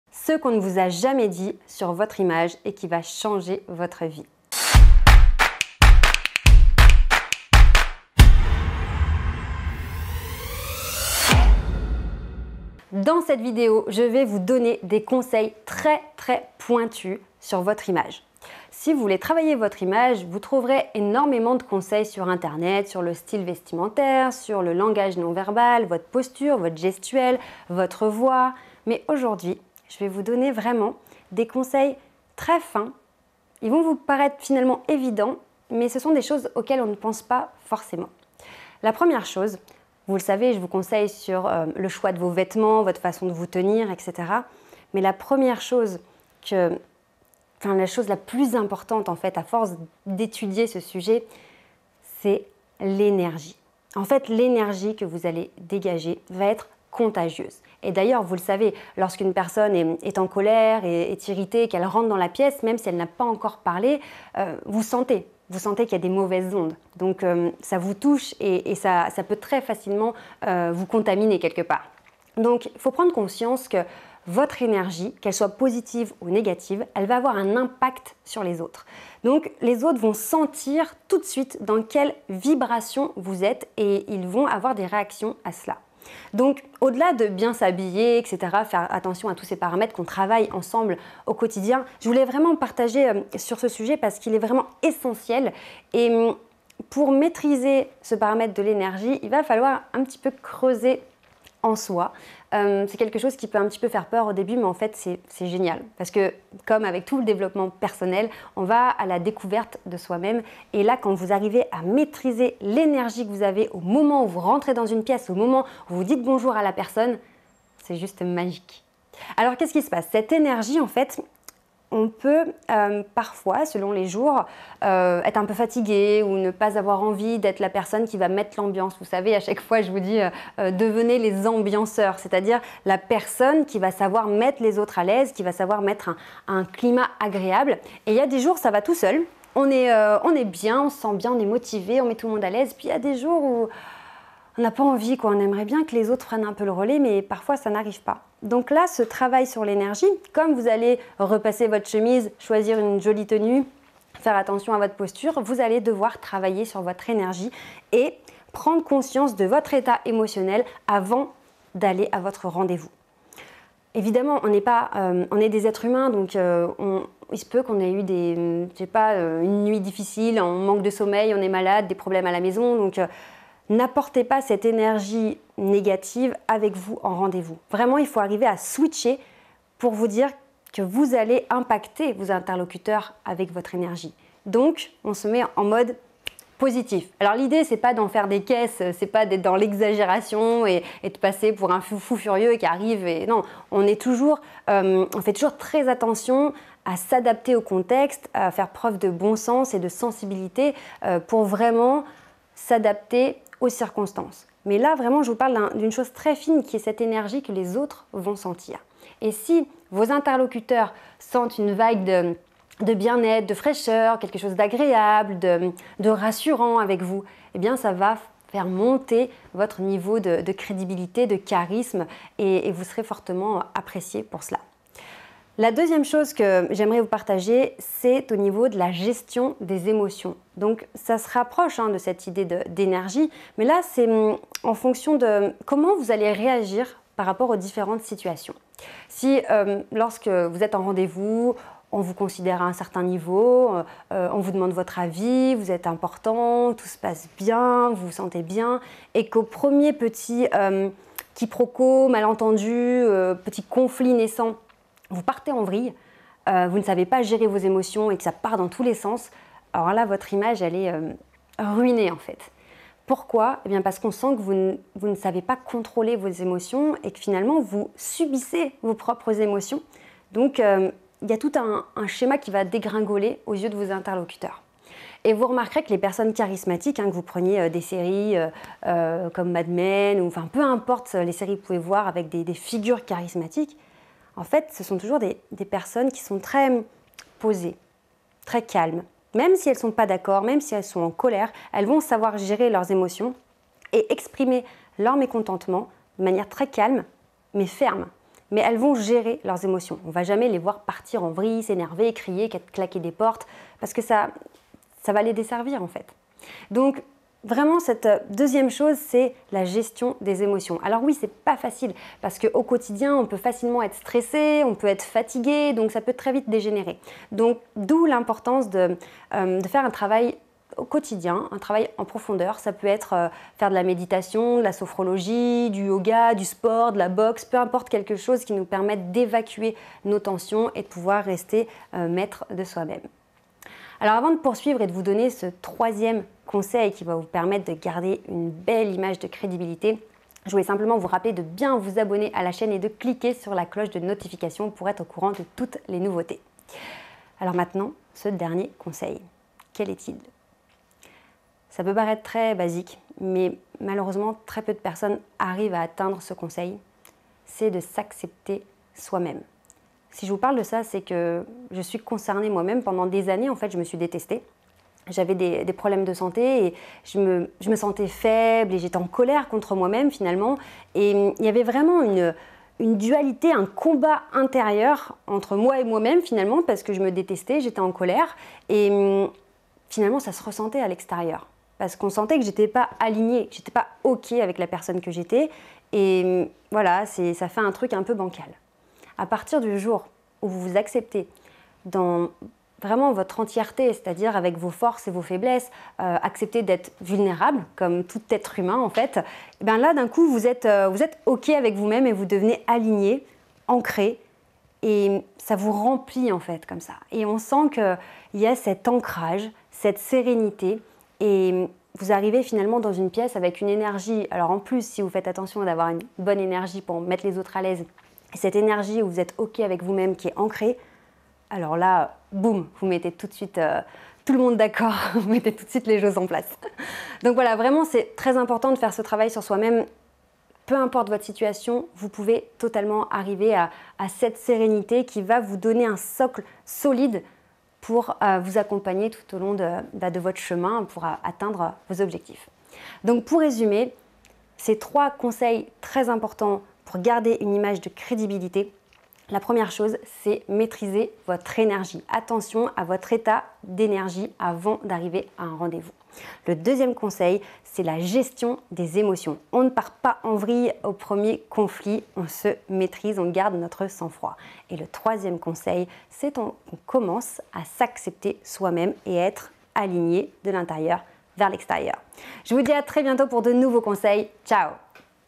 Faire bonne impression : ce qu’on ne vous a jamais dit sur votre image !
Lorsqu’on parle d’image, on pense souvent au style vestimentaire, et parfois à la communication non-verbal. Certes ces 2 paramètres sont importants, il est fondamentale de les maitriser mais ils ne font pas tout !

⭐ | Découvrez la méthode STAIR pour devenir plus Charismatique en cliquant sur le lien ci-dessous :

Au delà des vêtements et des gestes, quel est le paramètre qui fera vraiment la différence ?

1 - Votre énergie fera TOUTE la différence ! Votre vibration, tous ces éléments qui sont impalpables mais tellement importants. C’est pour cette raison que certaines personnes sont rayonnantes et on ne saurait expliquer pourquoi. D’où l’importance de toujours se préparer avant un rdv pour être dans de bonnes conditions et ...rayonner !

2 - Pour faire une bonne impression et avoir une bonne image, il sera extrêmement important de gérer vos émotions : stress mais aussi colère. Ne pas être en réaction face aux évenements qui pourraient vous destabiliser, mais rester dans le contrôle. Vous montrerez l’image rassurante d’une personne qui a confiance en elle et qui sait se maitriser.

3- Acceptez vous tel que vous êtes. Vous pourrez faire des améliorations grâce au conseil en image mais si vous ne vous acceptez pas en tant que personne, cela se sentira dans votre manière de communiquer. C’est lorsque vous êtes en paix avec vous-même que votre image devient réellement inspirante. Soyez bienveillant avec vous-même !

Vous n’aurez plus peur d'être vous-même, vous deviendrez authentique, et pourrez faire votre place pour atteindre la vie de vos rêves ! Devenez à l’aise partout, tout le temps et avec tout le monde !